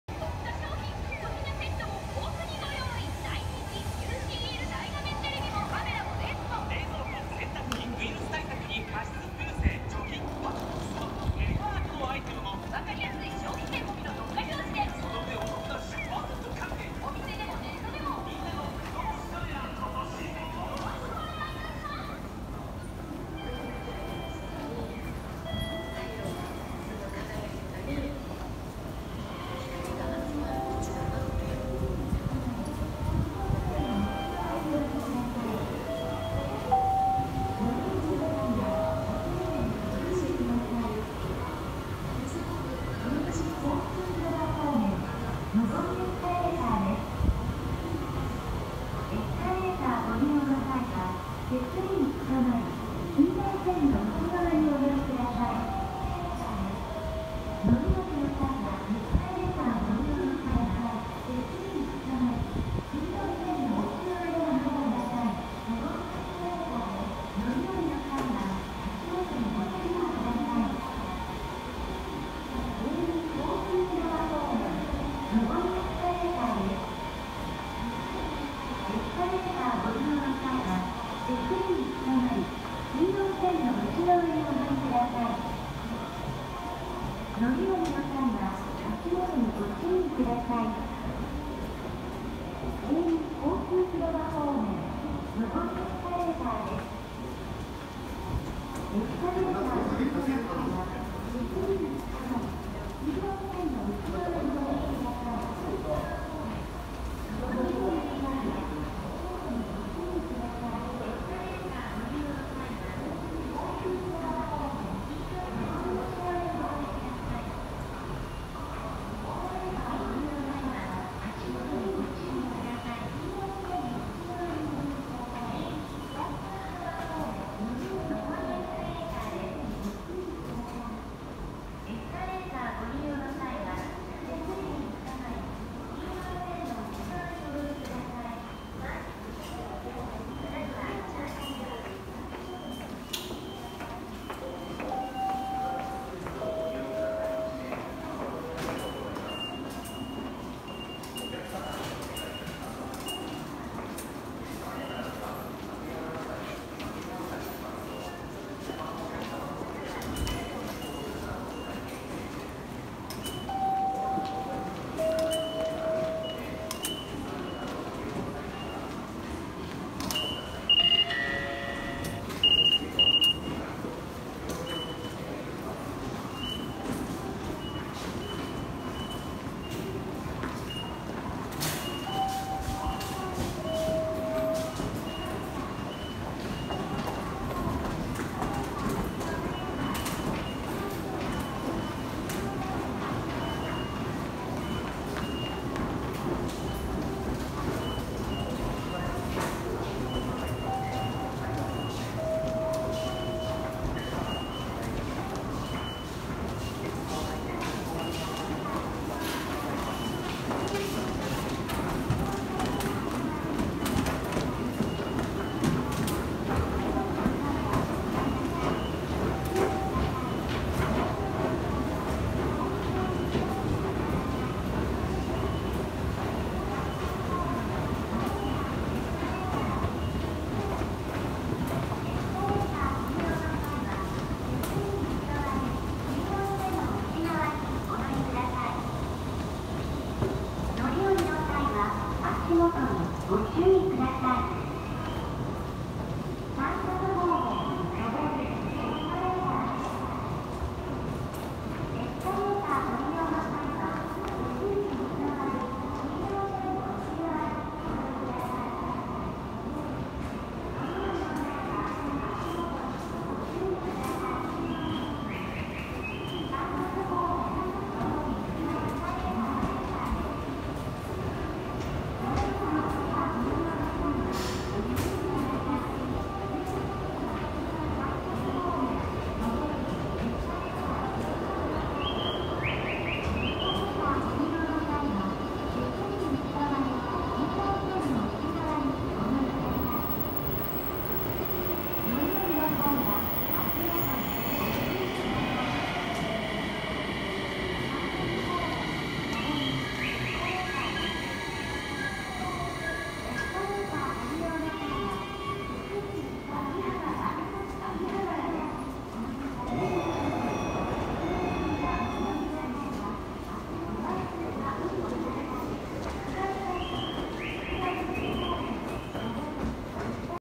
Tokyo subway, Tsukuba express line, Akihabara Station
In this video, I show the Tokyo subway train system. The name of the station is Akihabara station and this is the Tsukuba express train. I hope you will enjoy it.